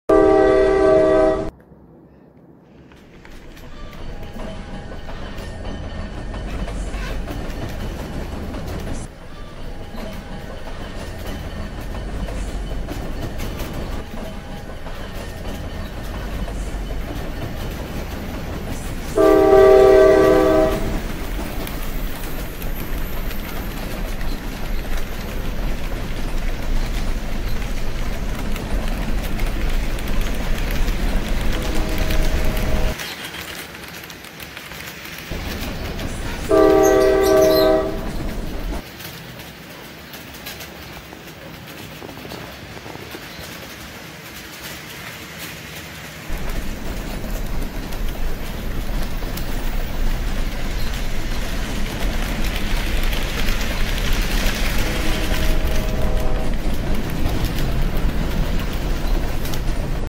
Different Types of Freight Cars on Track / Diesel Locomotive ** HO Scale Model Railway layouts **
Hey guys,
Happy New Year! Stay blessed & Stay safe.
As an upgrade from my previous video, I have added different types of freight car into this video.